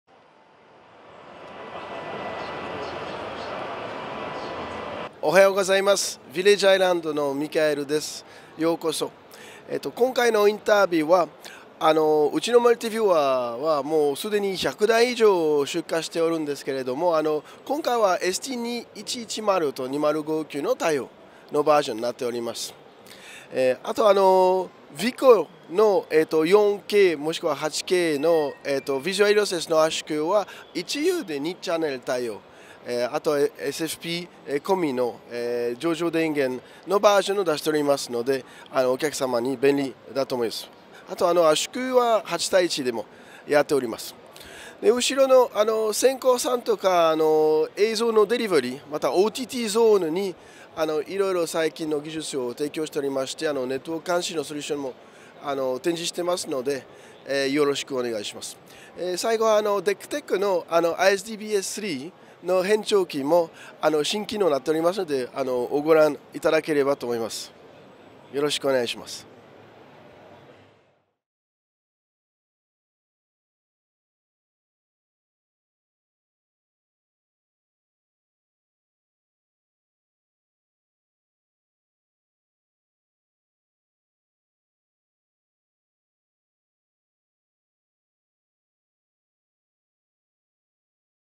【Inter BEE 2019 TV】ヴィレッジアイランド ST-2110対応のマルチビューワー「FlexViewer」、TICOコンバータ「VICO-4L」などを出展
ヴィレッジアイランドはInter BEE 2019で、ST-2110対応のマルチビューワー「FlexViewer」、TICOコンバータ「VICO-4L」、4K デコーダ「SENCORE」、ISDB-S3 変調機「Dektec」を出展する。

映像制作/放送関連機材部門 / ホール5 / 小間番号5502